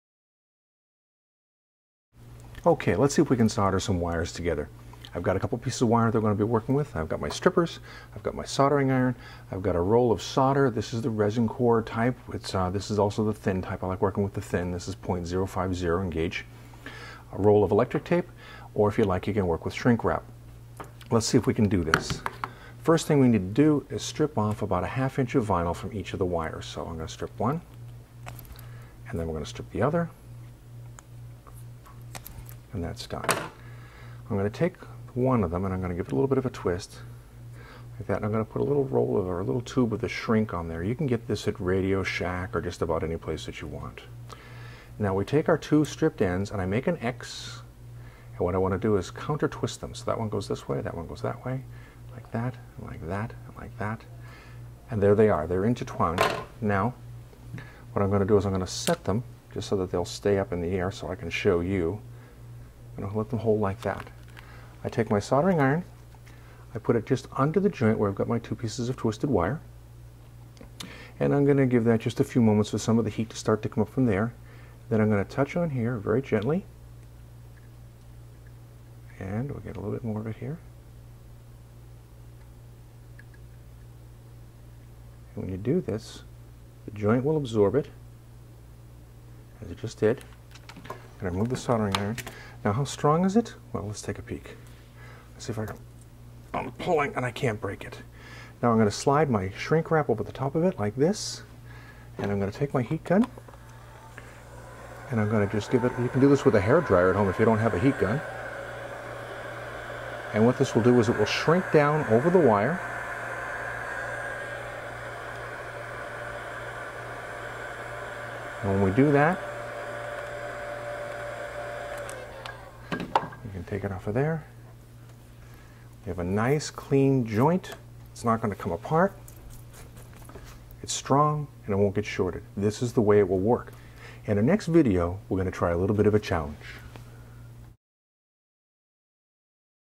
Soldering Wire Connections (Part 2) - How to do it Properly
Moss Motors Tech shows you how to properly solder wire connections. It is simple and the best possible method for connecting two wires.